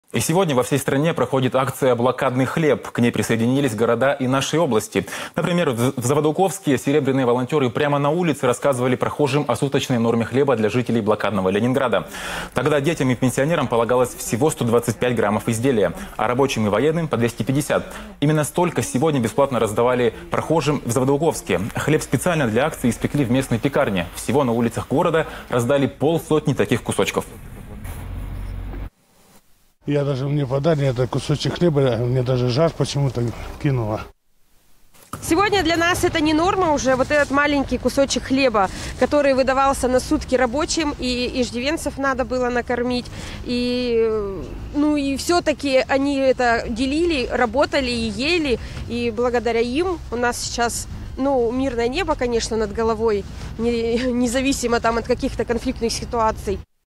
Заводоуковск присоединился к Всероссийской акции "Блокадный хлеб"
27 января во всей стране проходит акция "Блокадный хлеб". К ней присоединились города и Тюменской области. В Заводоуковске серебряные волонтёры прямо на улице рассказывали прохожим о суточной норме хлеба для жителей блокадного Ленинграда. Тогда детям и пенсионерам полагалось всего 125 граммов изделия. Рабочим и военным—- по 250. Именно столько сегодня бесплатно выдавали и прохожим в Заводоуковске. Хлеб специально для акции испекли в местной пекарне. Всего на улицах города раздали полсотни таких кусочков.